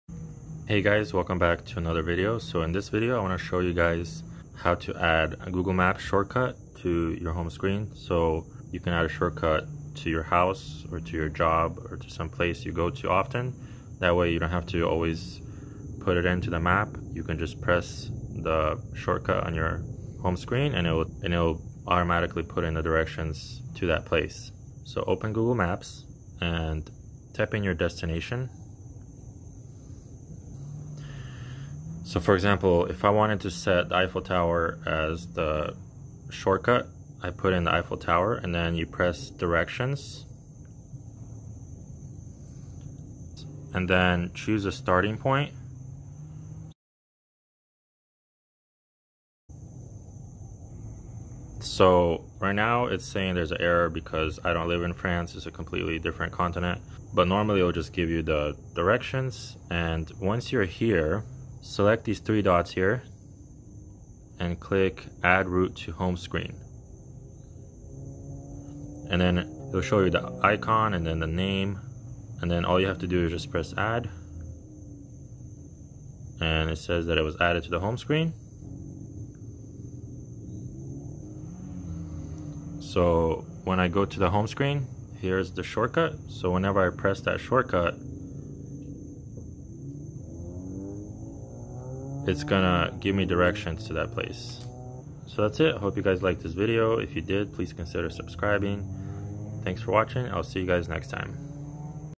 Create Google Maps Shortcuts To Home, Work, Etc
This video explains how to create Google Maps shortcuts. You can create a shortcut to home, work or any place you go often to save you the time of putting in the address every time.